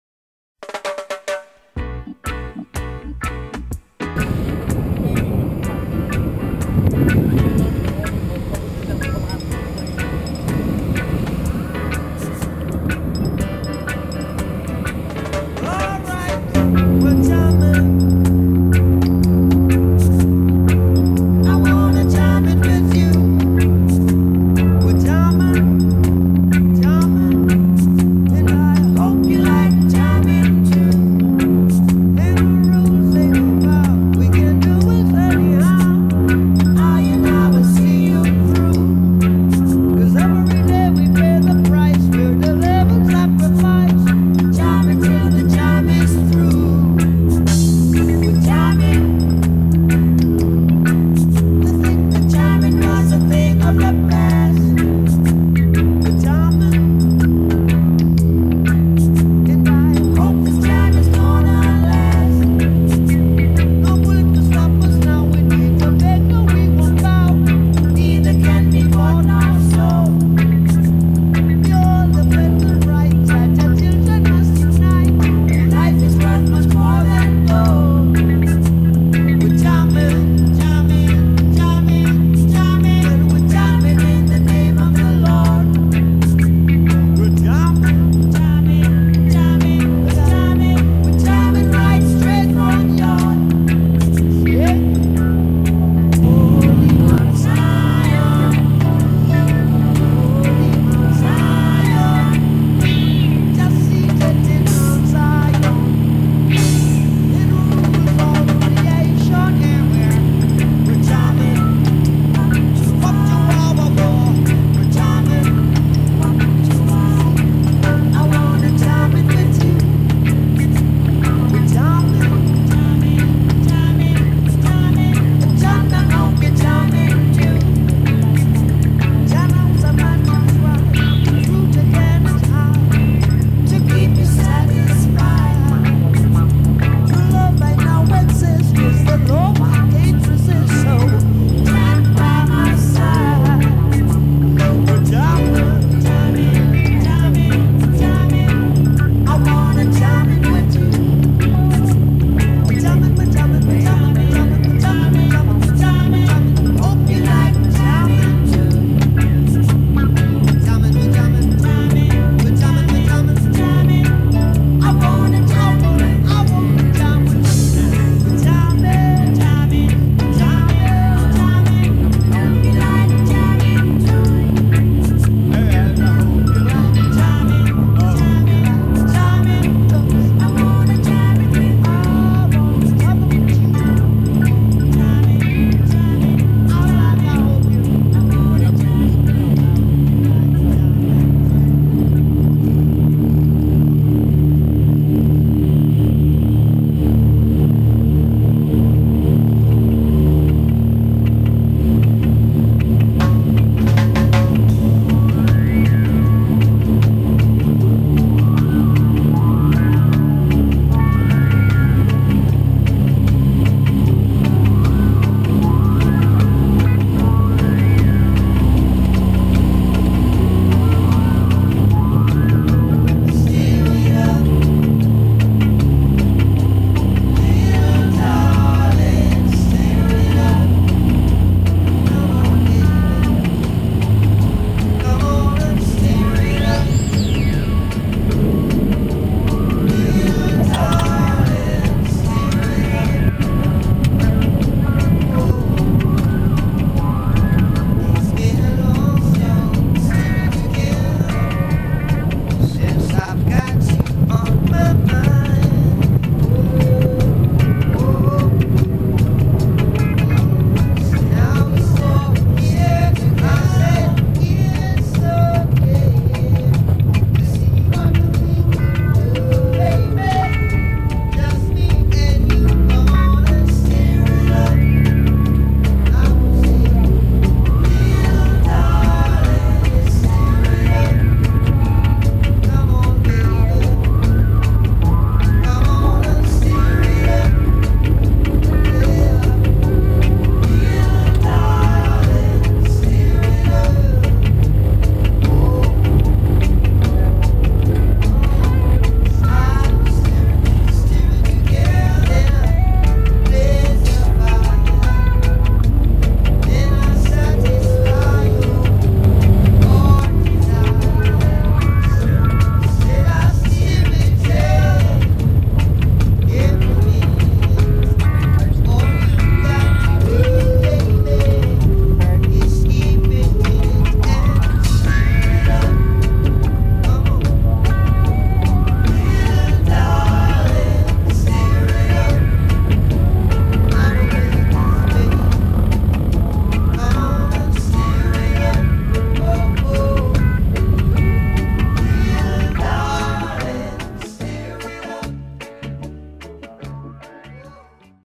Roatan to Utila.wmv
Flying home to Utila from Roatan with Captain Angelo of Island Air